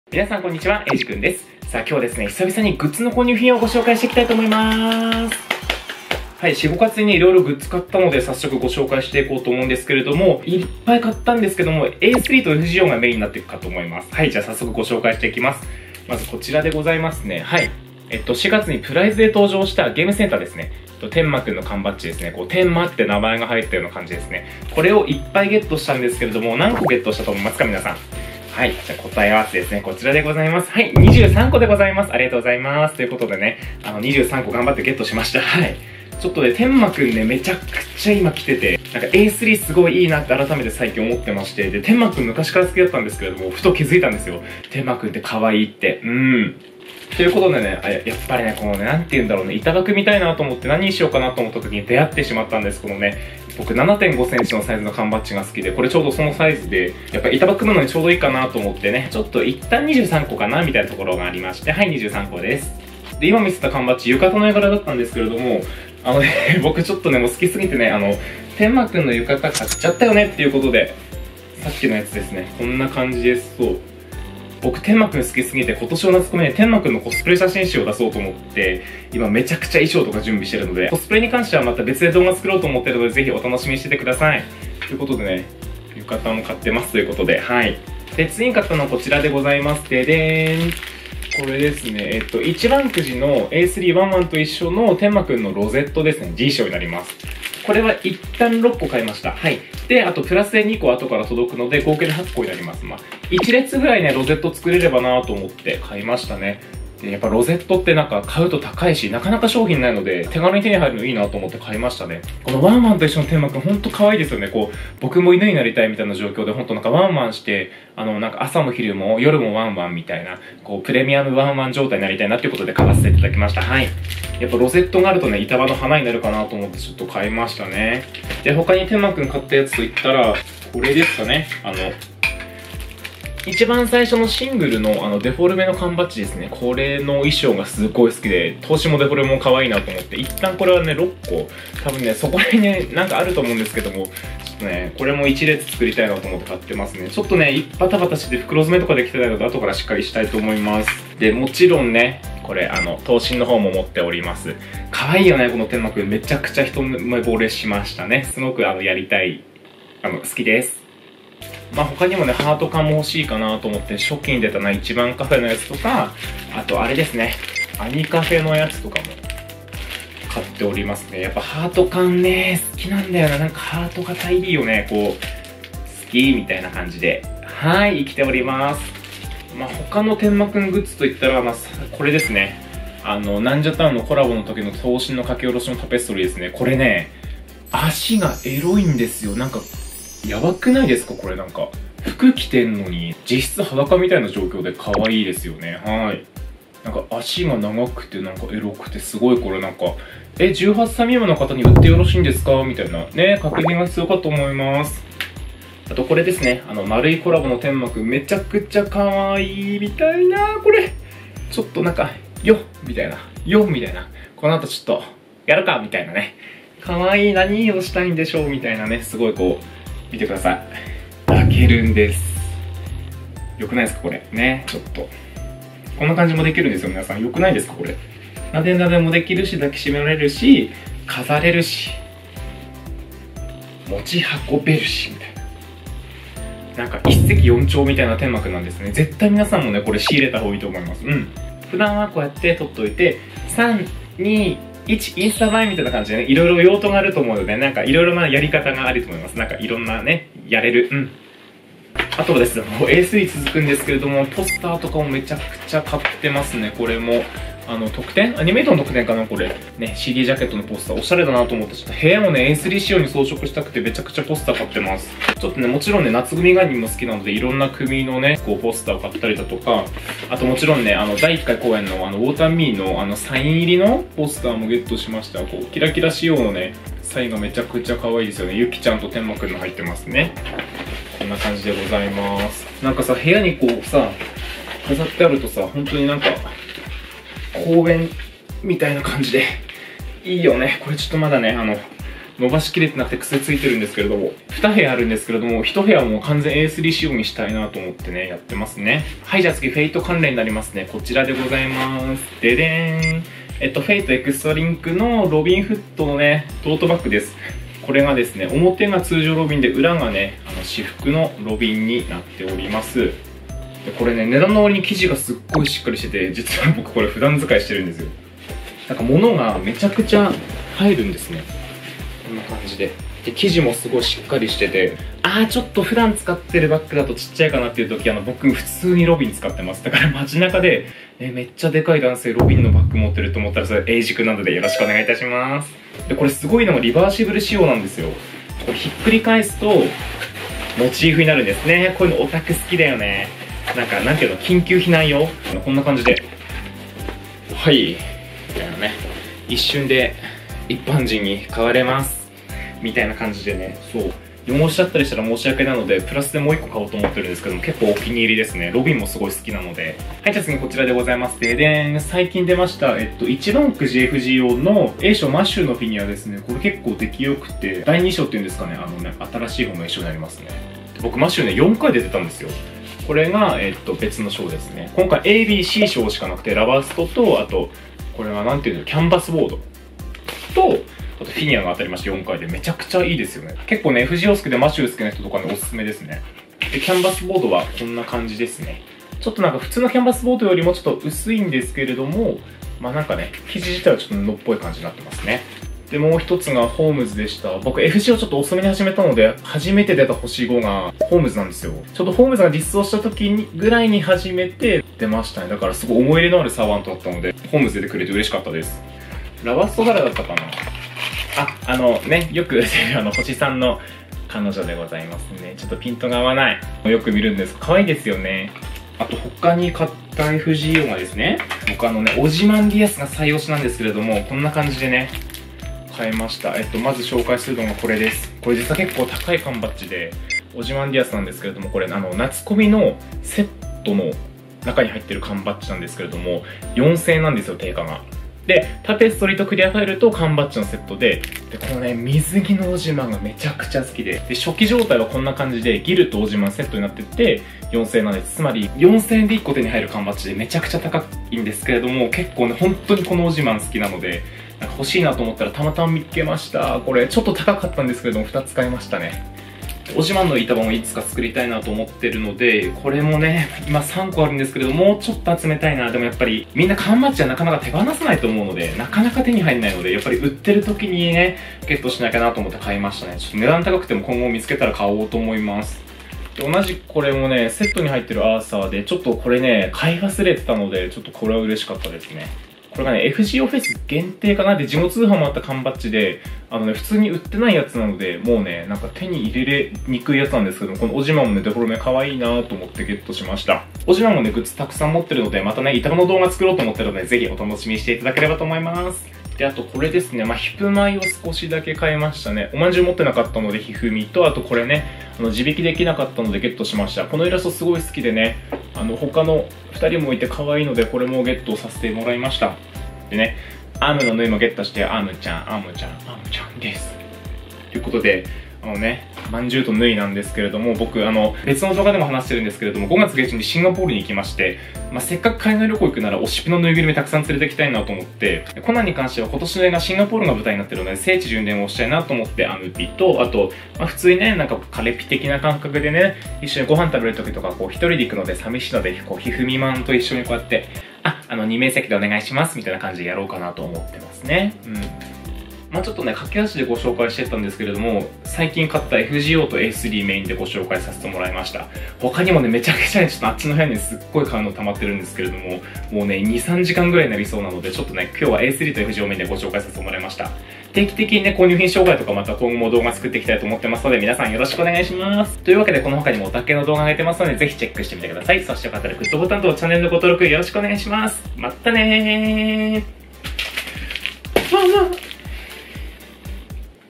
【A3!】イケメン好きオタク男子の推しぴグッズ爆買い購入品紹介!!★【FGO】
※FGOラバストのマタ・ハリちゃん、★3じゃなく★1でございました…！！
言い間違えてしまいすみませんっ！！(T_T)